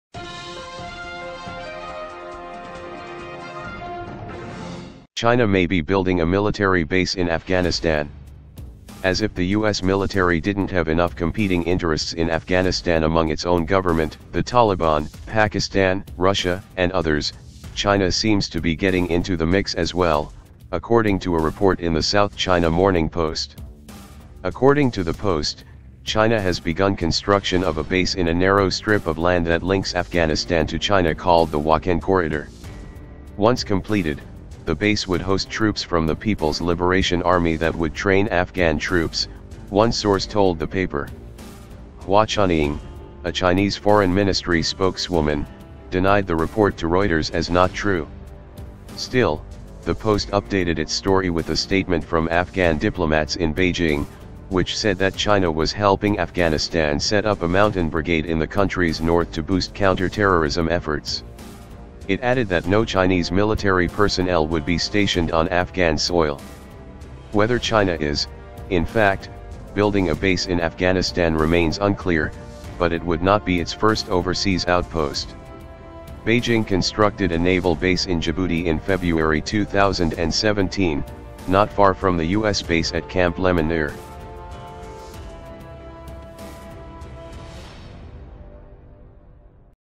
China May Be Building A Military Base In Afghanistan
As if the U.S. military didn’t have enough competing interests in Afghanistan among its own government, the Taliban, Pakistan, Russia, and others, China seems to be getting into the mix as well,  according to a report in the South China Morning Post.

According to the Post, China has begun construction of a base in a narrow strip of land that links Afghanistan to China called the Wakhan Corridor. Once completed, the base would host troops from the People’s Liberation Army that would train Afghan troops, one source told the paper.